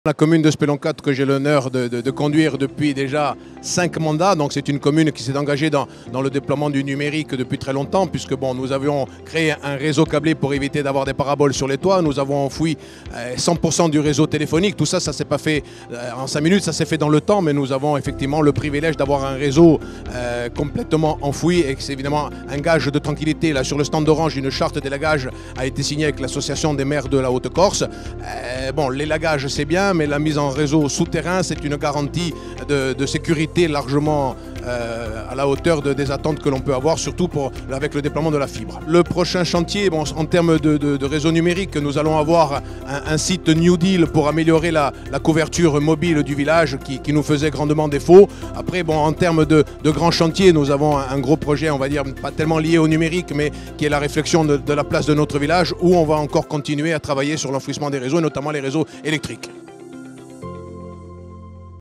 Focus sur Speloncato avec Jean-François Poli
A l'occasion du Congrès des Maires qui s'est tenu du 21 au 23 novembre dernier Porte de Versailles, les élus Corse sont venus en force. Une des séquence importante de ce déplacement fut la signature d'une convention élagage sur le Stand Orange. Un travail de fin important pour la résilience des réseaux alors que les conditions climatiques de l'Ile de Beauté peuvent mettre à mal la qualité de connexion. A Speloncato, les élus ont même fait le choix d'enterrer la fibre. Pour en savoir plus, nous avons pu poser quelques questions à son maire, Jean-François Poli.